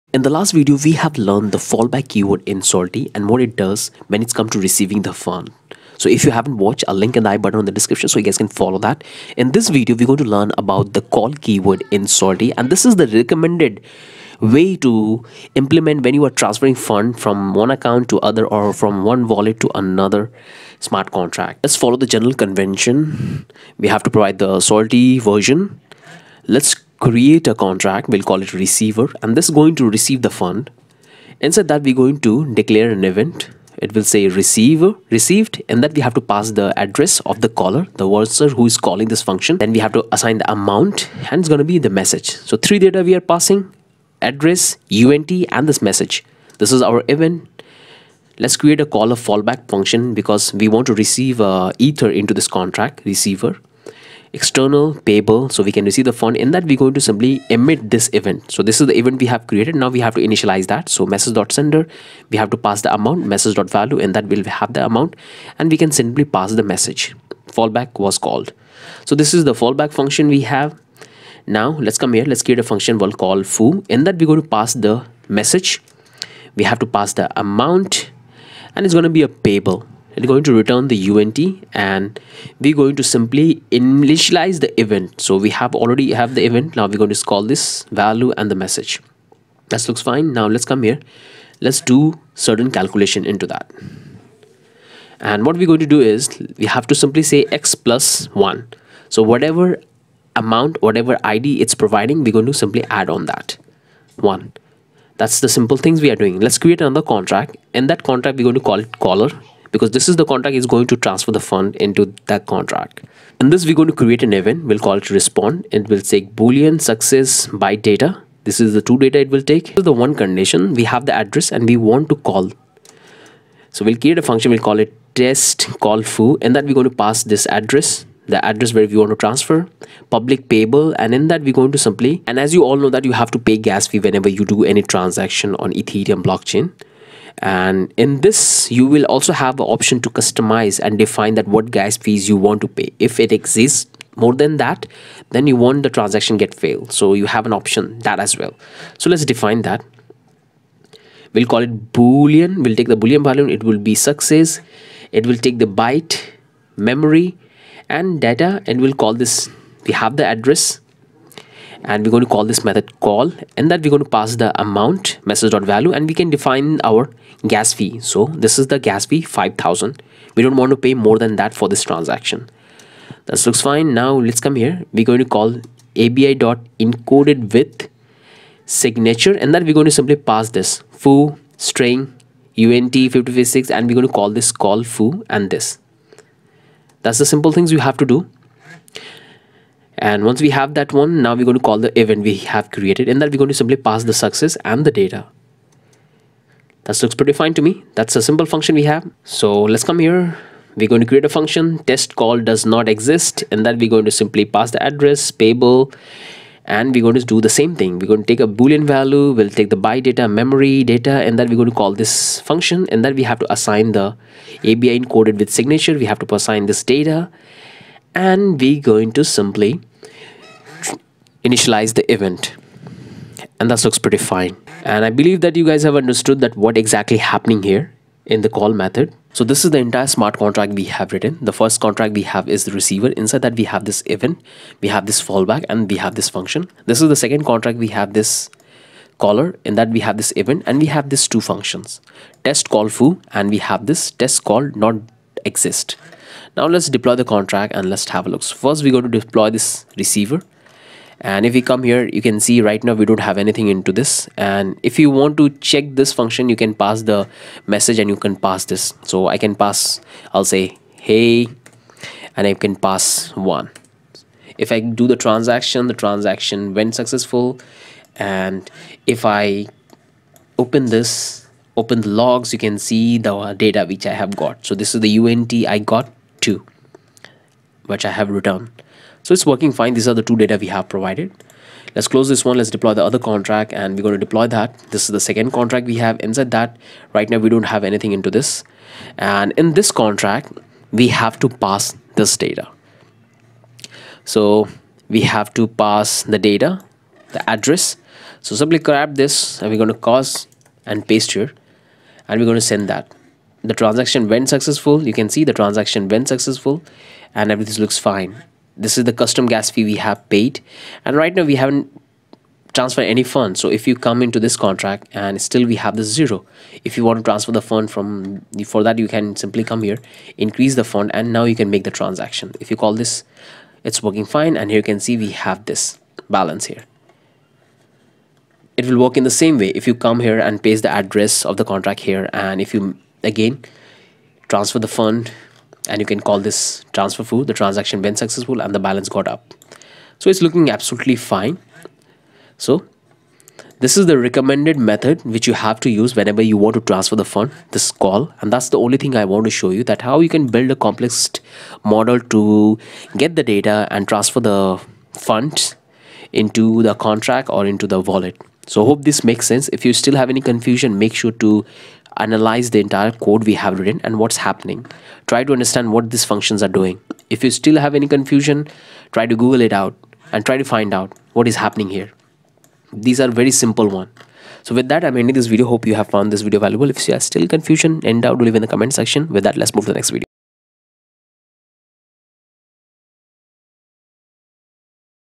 Solidity Call Function | How To Create & Use Call Function In Solidity | Solidity Course 2022
Solidity Call function

call is a low level function to interact with other contracts.

This is the recommended method to use when you're just sending Ether via calling the fallback function.

However it is not the recommend way to call existing functions.

Workout Video: